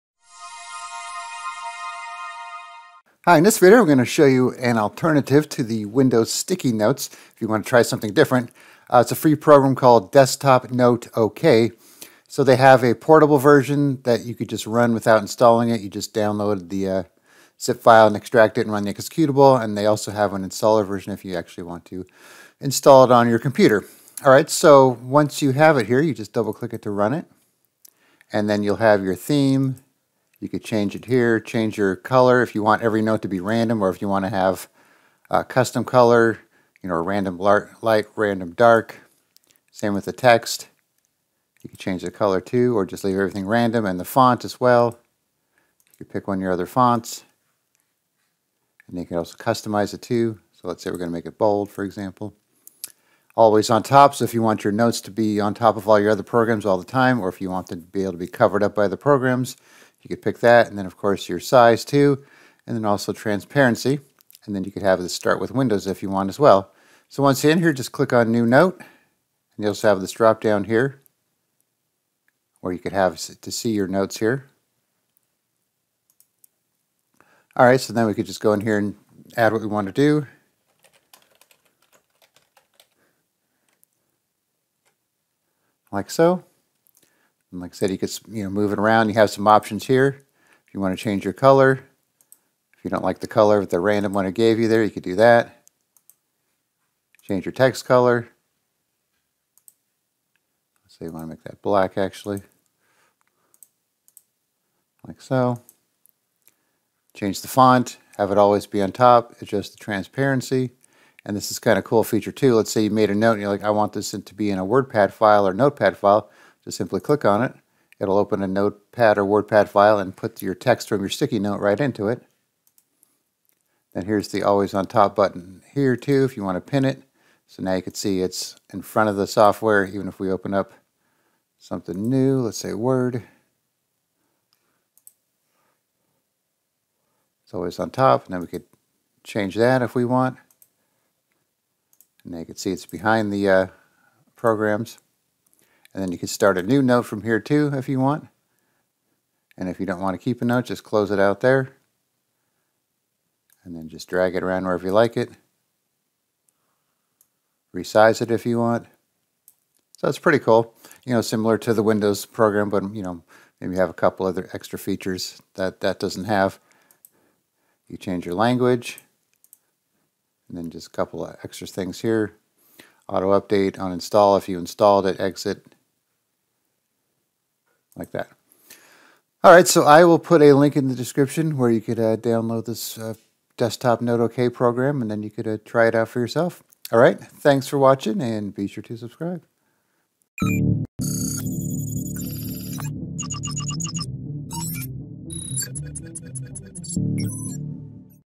Desktop Note OK - Alternative for the Windows Sticky Notes
Windows comes with its own post it note software called Sticky Notes that you can use to make notes on your desktop. If you want to try another note taking app that is free and easy to use, then you might want to check out Desktop Note OK. With this app, you can customize note and text colors, fonts, transparency, size and whether or not the notes are on top of other programs. When you close a note, you can then reopen it from the program itself if you need to see it again.

Here is a link to download the Desktop Note OK software.

Check out our book (Computers Made Easy) that is designed to take your computer skills to the next level!